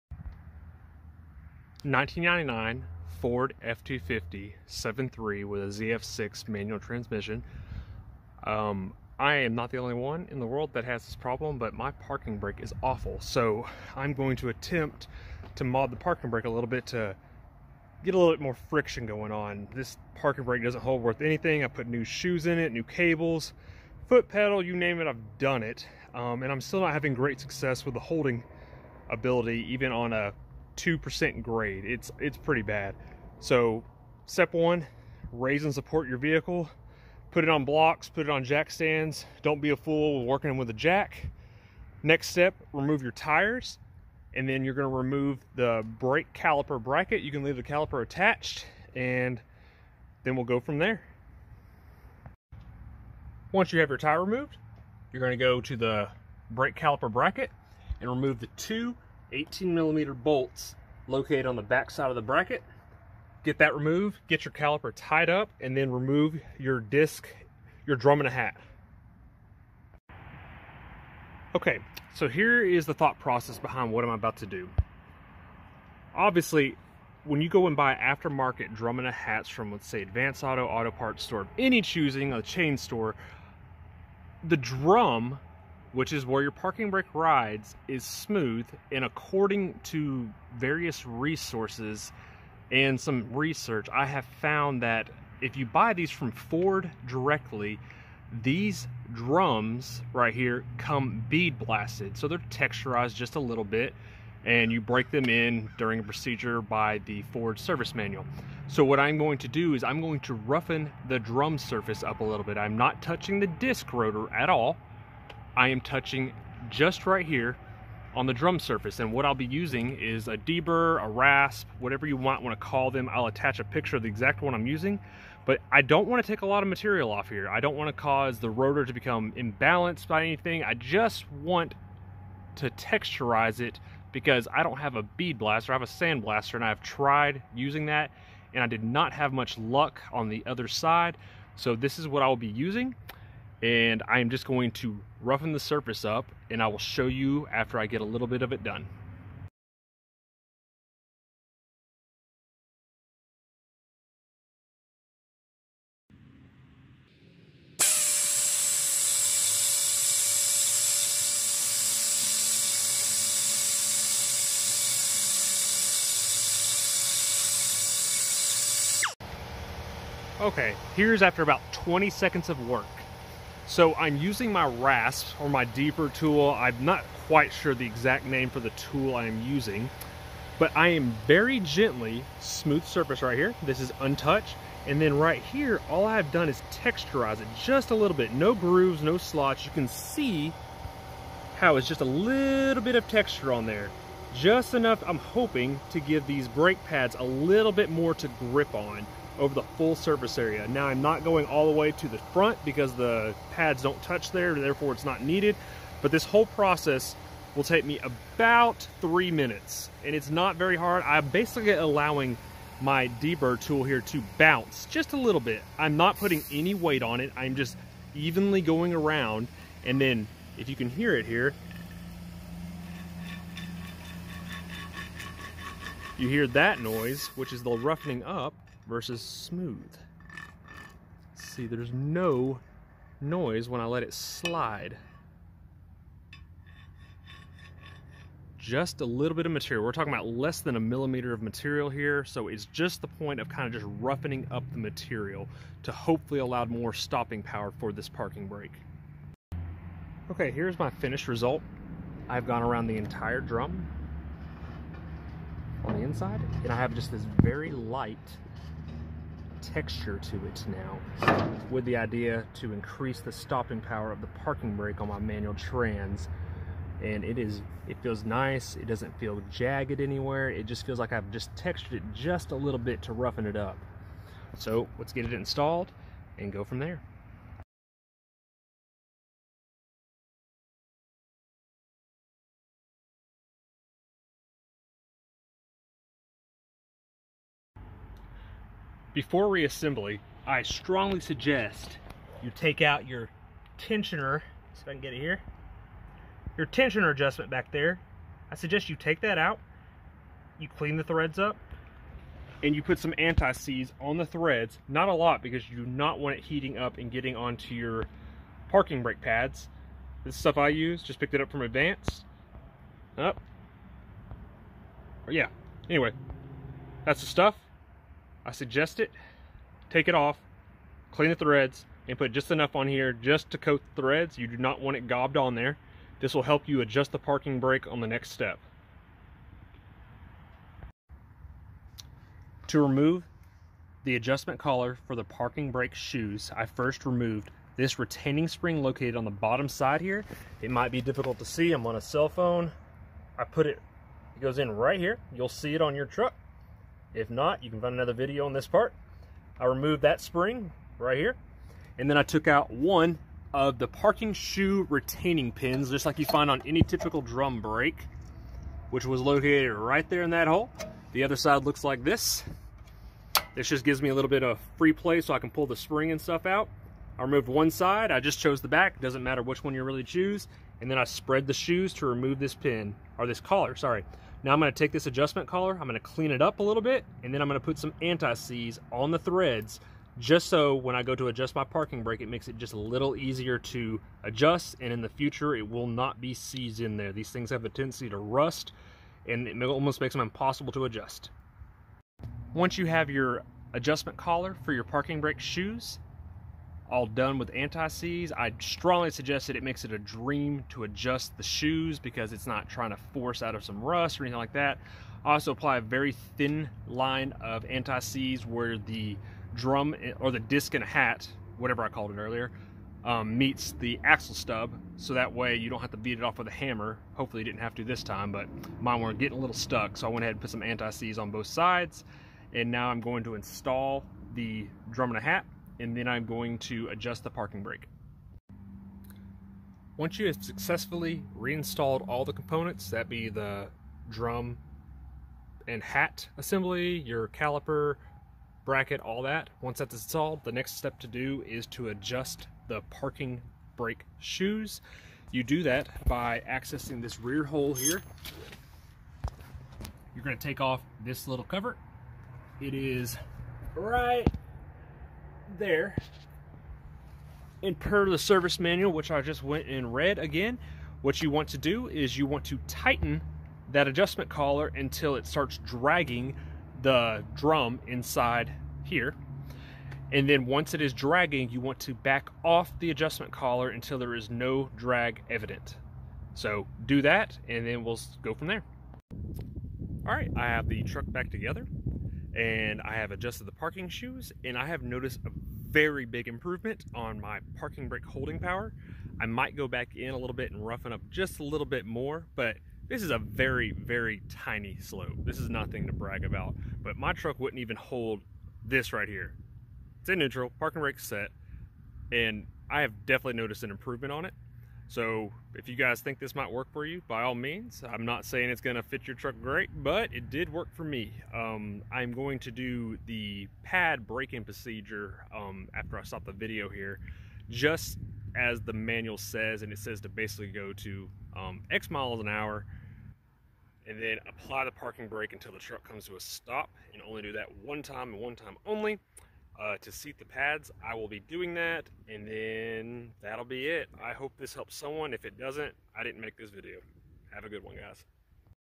1999 Ford F-250 ZF6 parking brake mod
This is my attempt at improving the parking brake for the manual transmission f250s. I haven't seen anybody else do this, but I am attempting to improve upon the original design of the Ford parking brake for the manual transmissions. Attempt at you own risk.